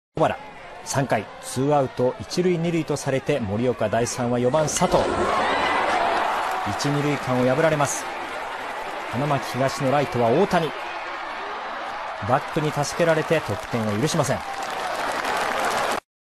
高校２年時の大谷のレーザービーム
大谷やっぱ
肩が強い！！